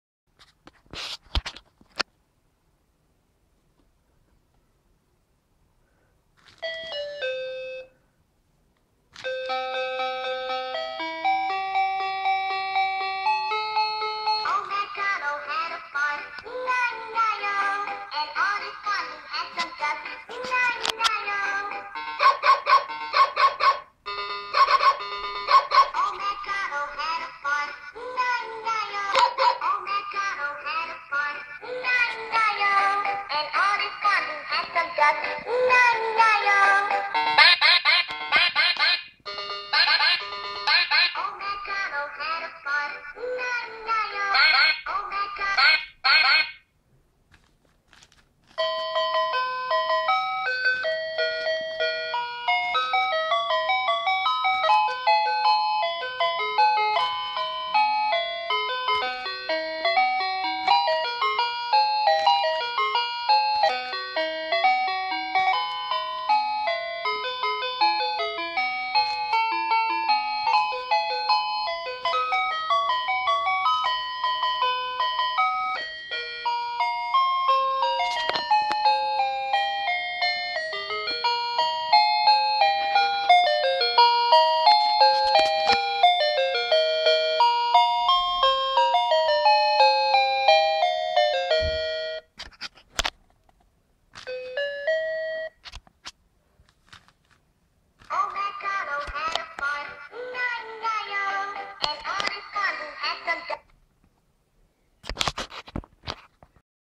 Zenélő bébijáték
Régióban kapható zenélő halacska formájú bébijáték